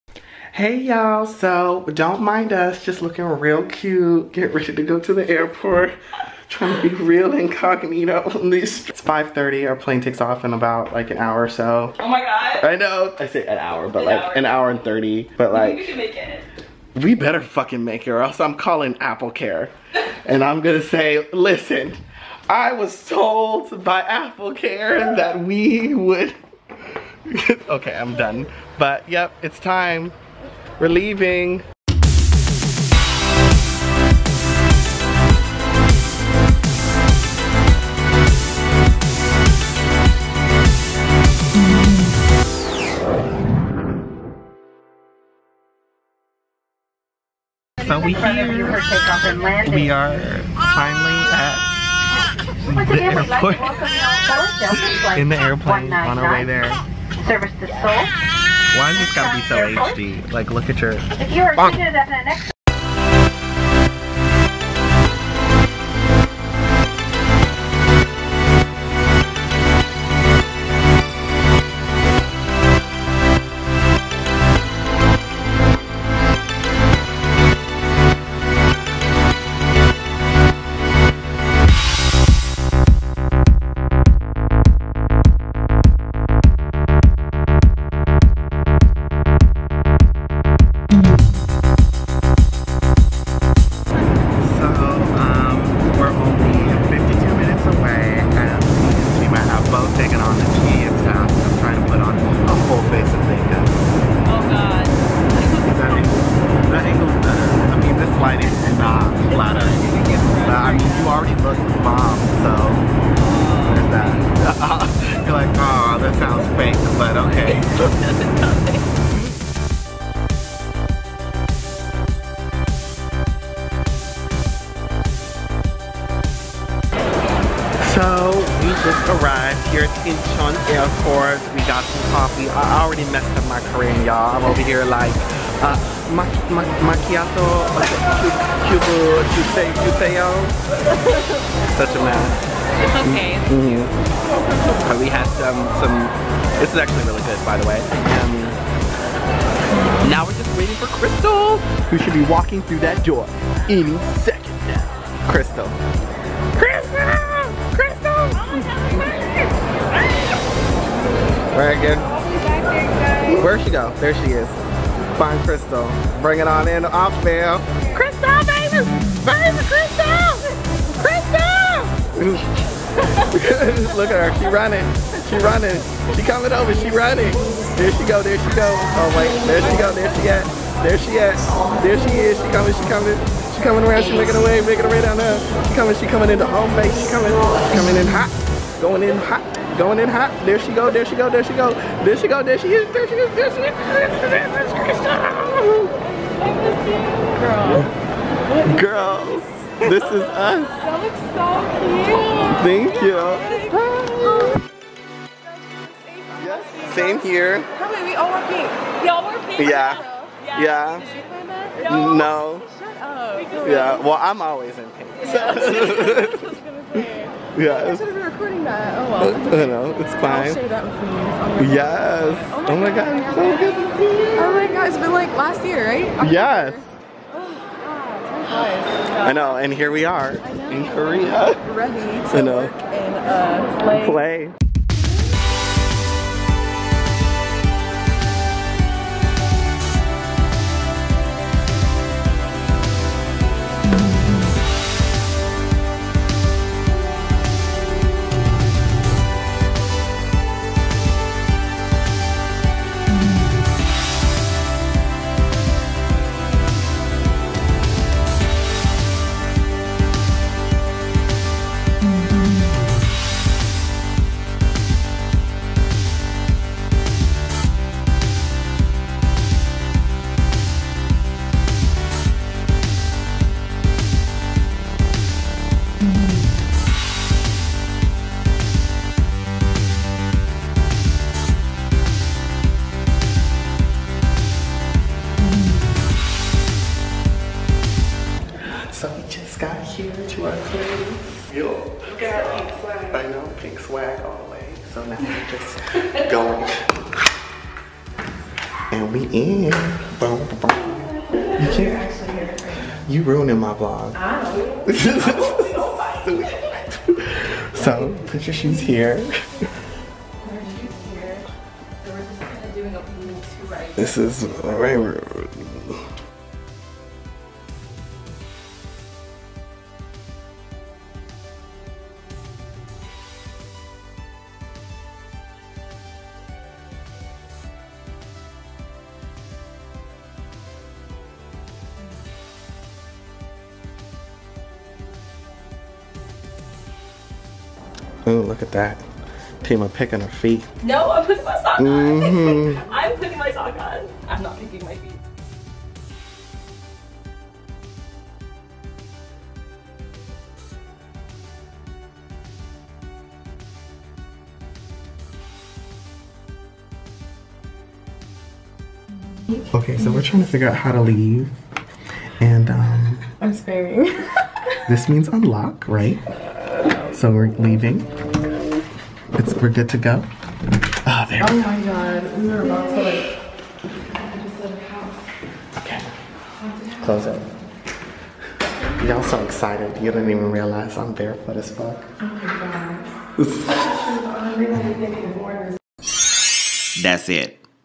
Miles Away: South Korea Day 1
In this epsiode, Tima and I lift off and land in South Korea, where we meet our best friend Crystal and begin to get settled in the big city of Seoul!!!